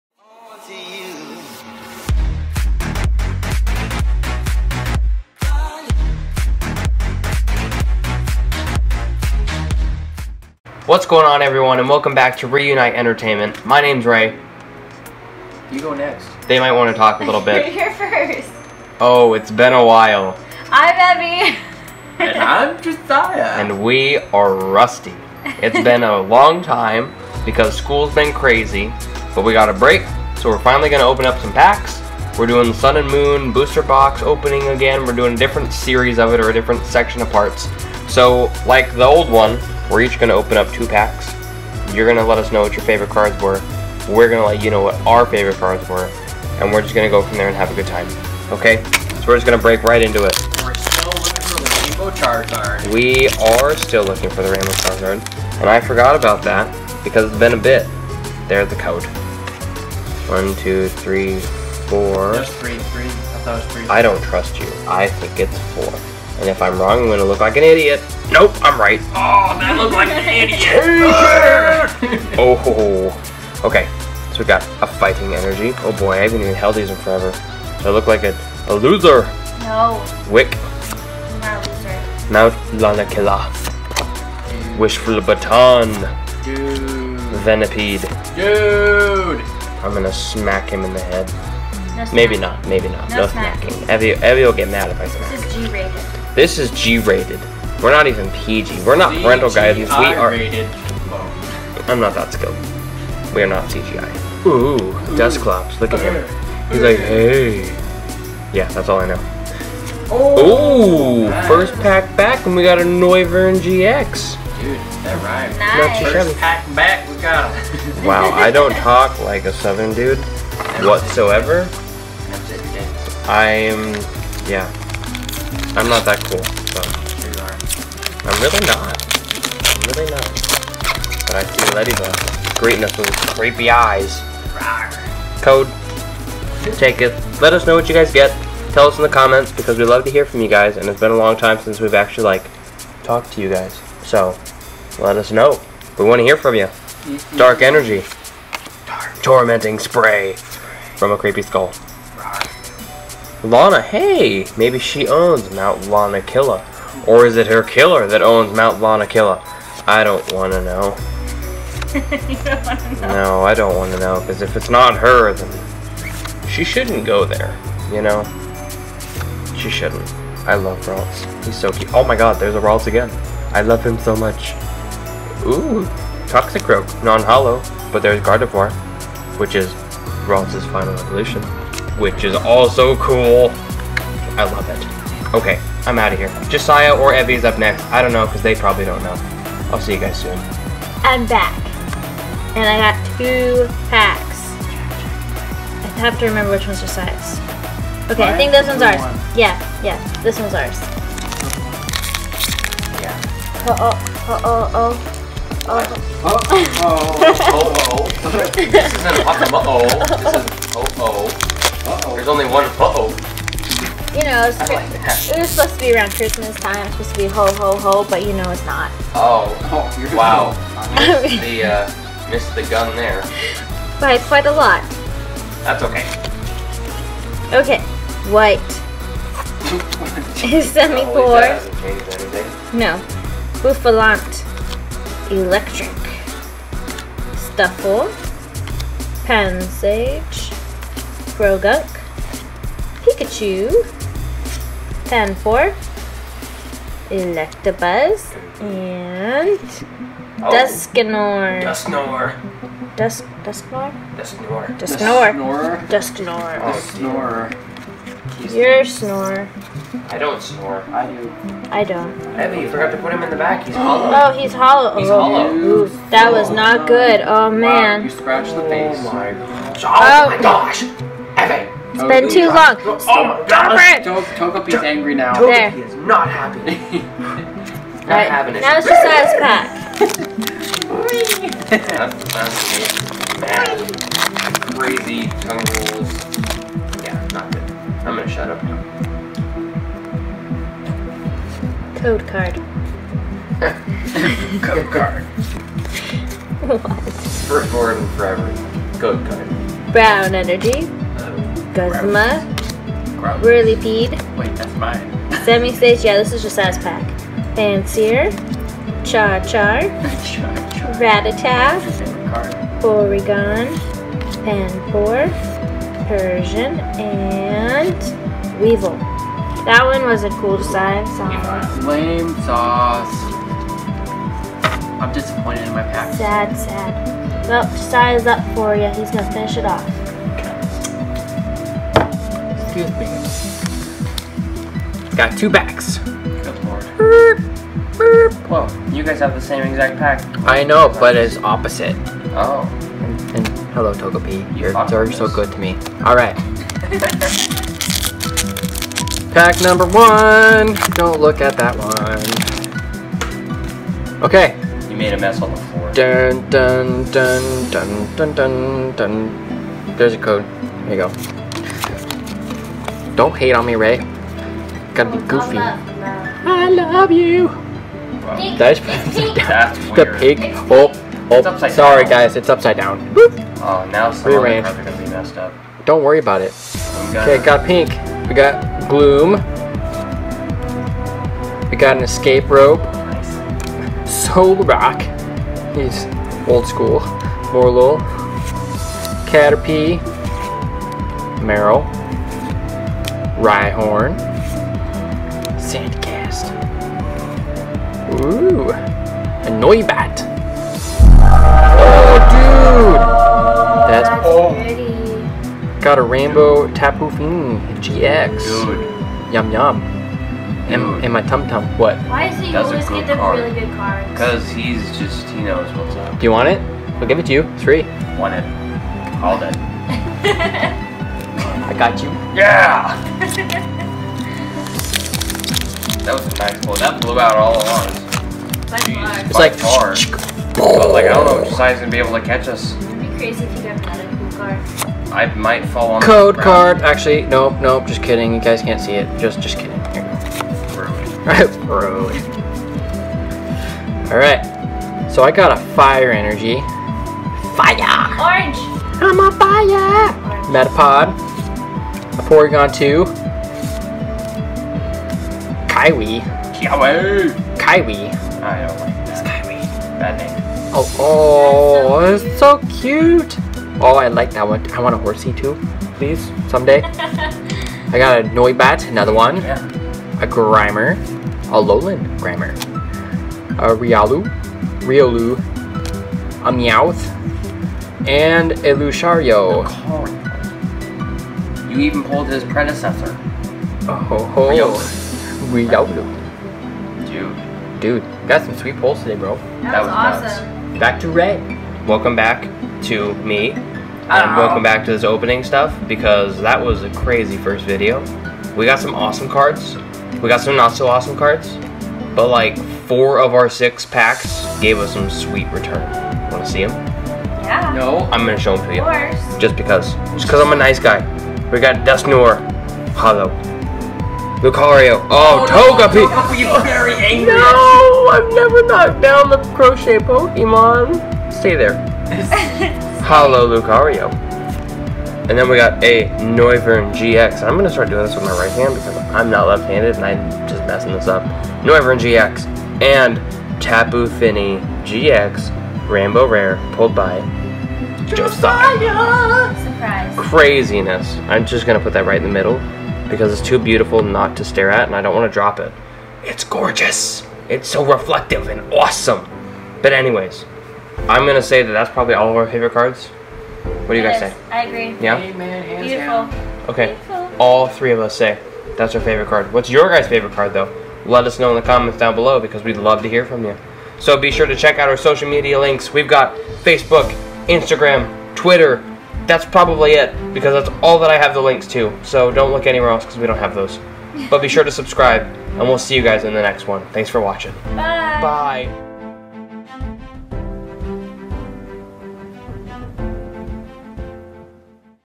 Sun and Moon Burning Shadows - Series: Part 1 (DID THAT JUST HAPPEN!?)
What's going on everyone, and welcome back to RE-Unite Entertainment! It's been a couple of months due to busy schedules, and we've definitely missed recording and uploading videos! But today we are continuing to open packs and attempt at making you all laugh :D Come along with us as we dive into some more Sun and Moon Burning Shadows packs and see what luck we can muster up! Let us know what your favorite card we pulled in this episode was! Your support means the world to us and we can't wait to bring you more great content! :D

Evie's Etsy Shop